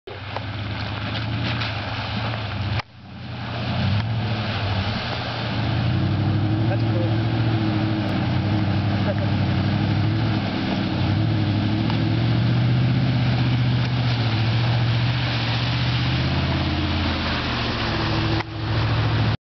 jeep water crossing 3 feet deep!
Ash enjoying her Jeep in the water